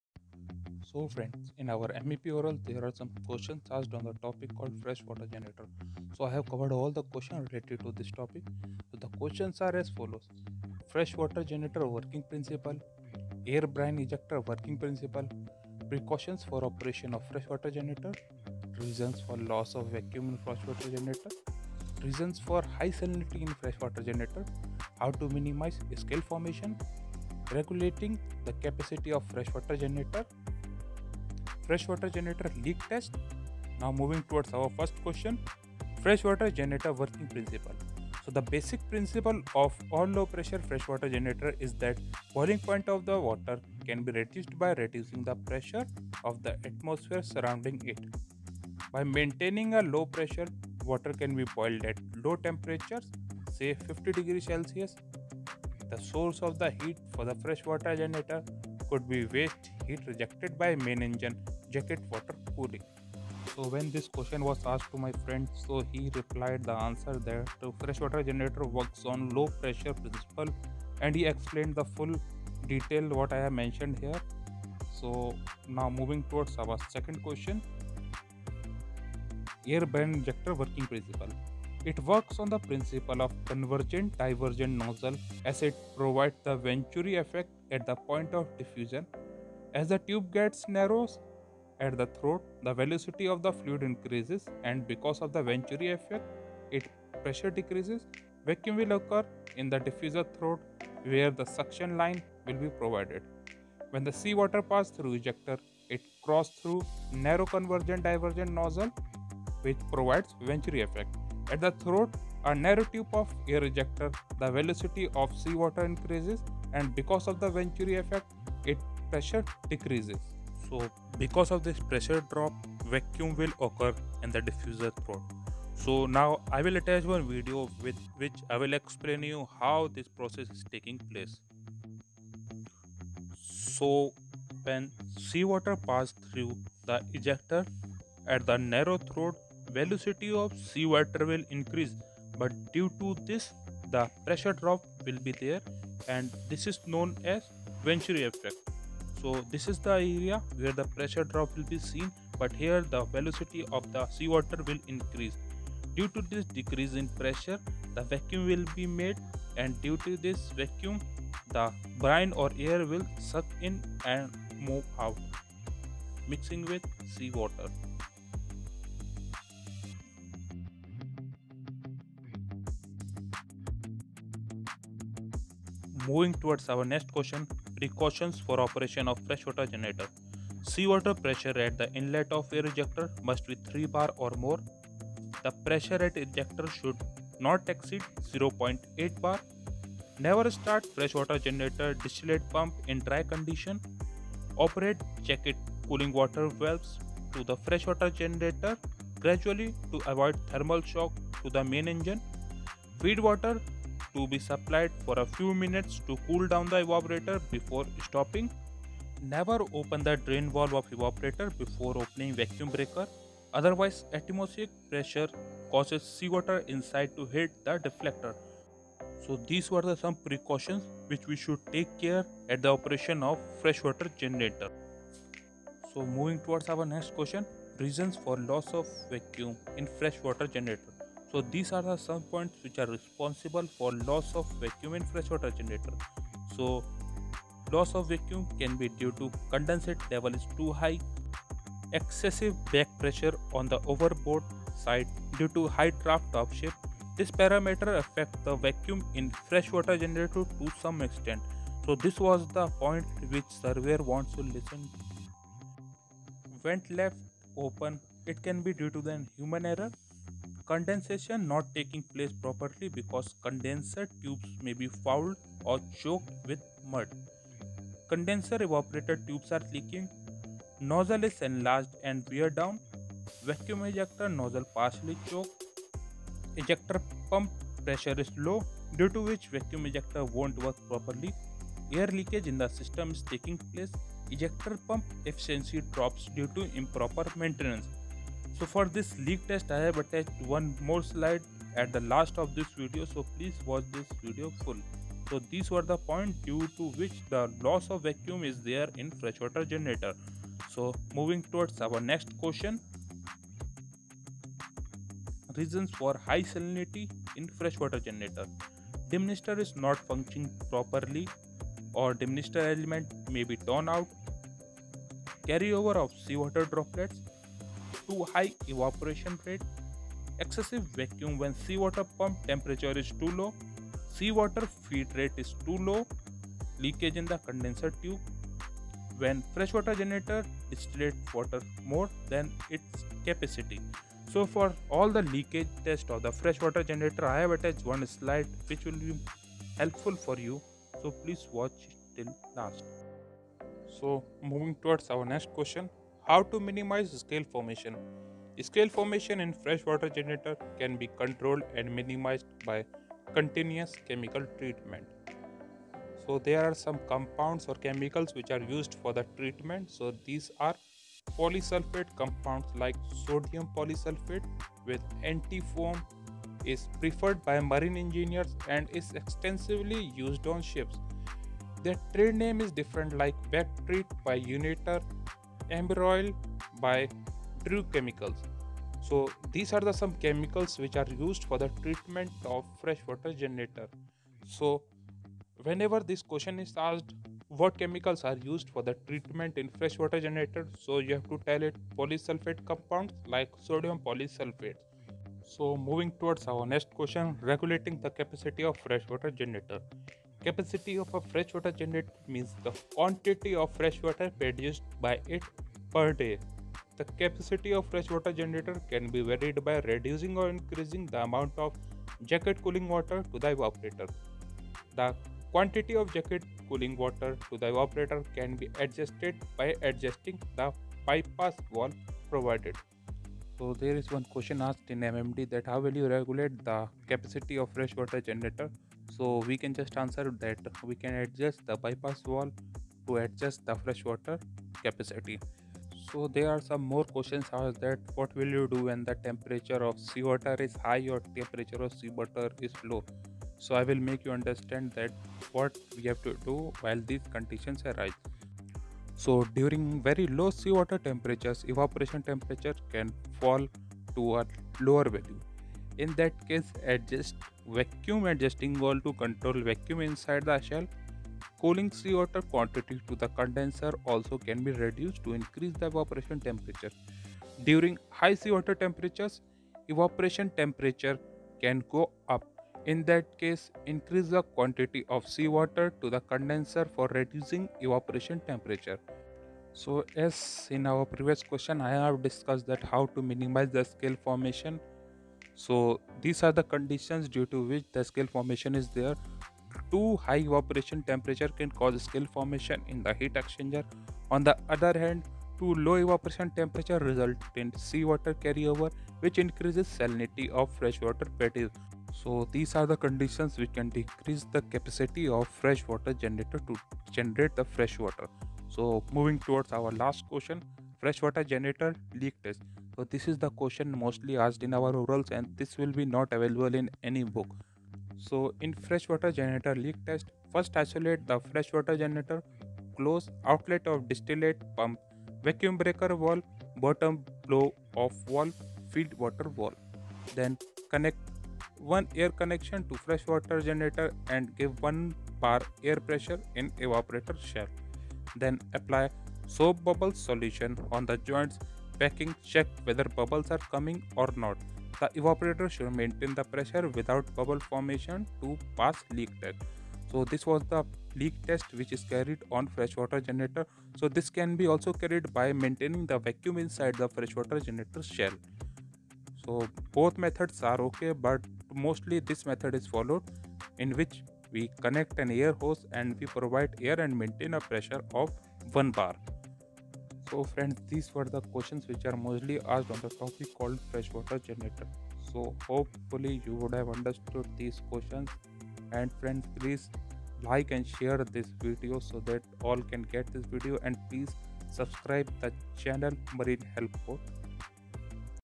FRESH WATER GENERATOR | FWG SALINITY | FWG VACCUM LOSS | FWG LEAK TEST | MEP ORAL |MEO CLASS 4 ORALS
MEP ORAL

Ques: FWG working principle ?

Ques: Air/Brine Ejector Working principle ?

Ques: Precautions for Operation of Fresh water Generator ?

Ques: Reason For Loss of Vaccum in FWG ?

Ques: Reason For High Salinity in FWG ?

Ques: How to Minimize Scale Formation ?

Ques: Regulating the Capacity of Fresh water Generator ?

Ques: FWG leak Test ?

                                                           Prepared By: SAHEEM KHAN